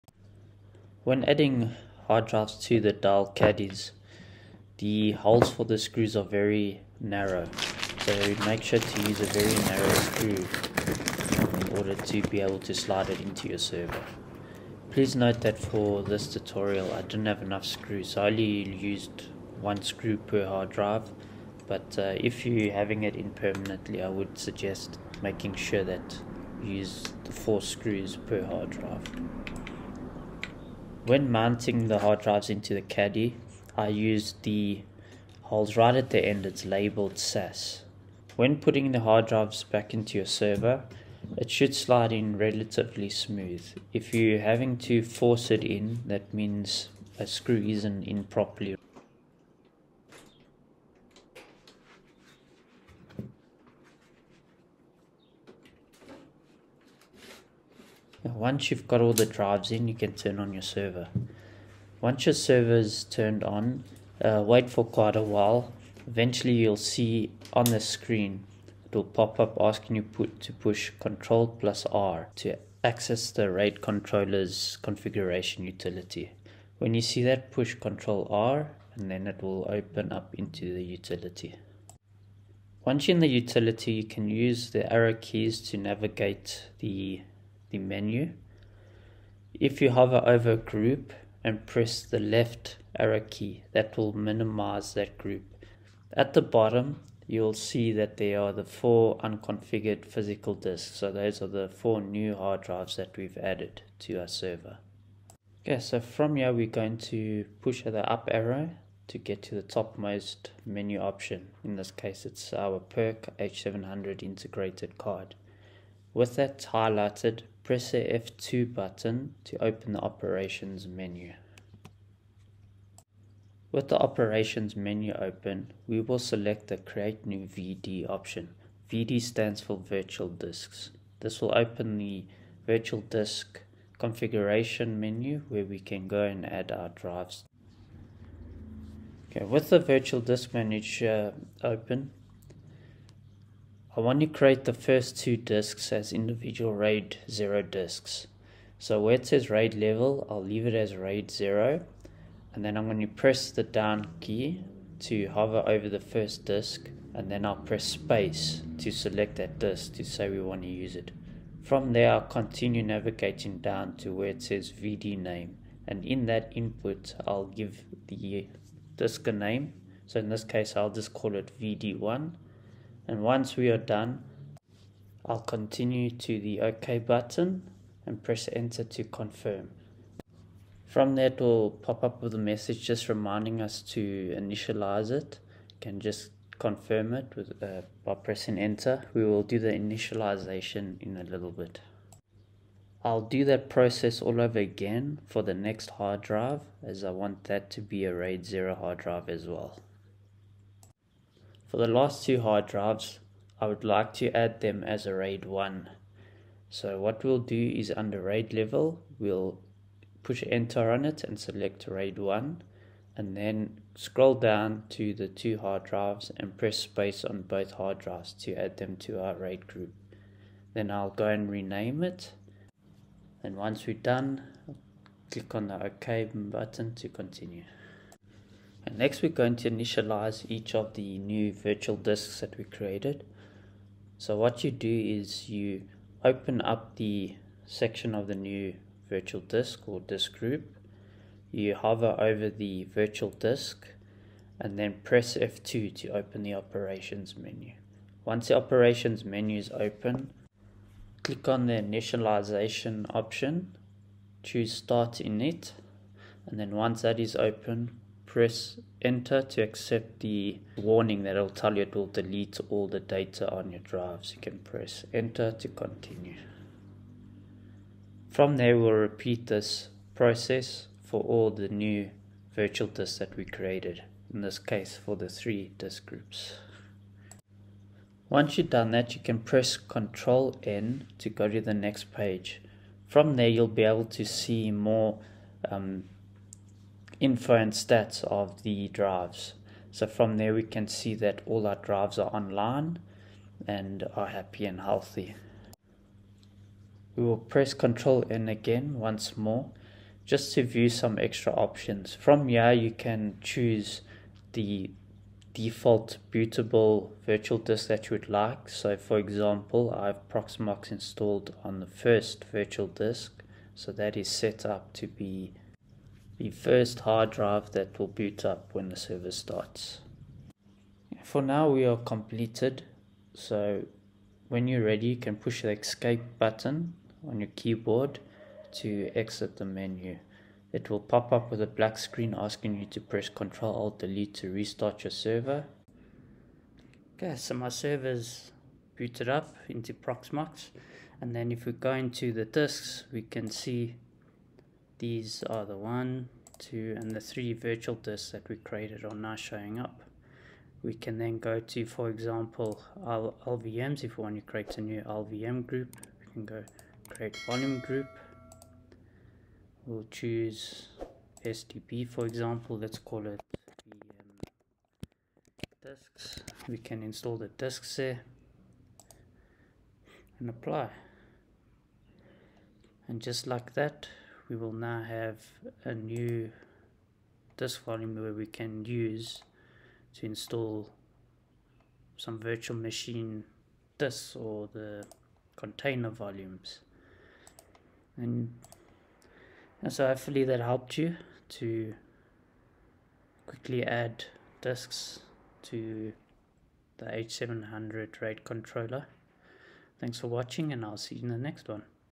How To Add Drives To The Dell PERC H700 RAID Controller
In this quick video I will show how to add HDDs and SSDs to your Dell PERC H700 RAID Controller. For a more detailed write up, go to the following link: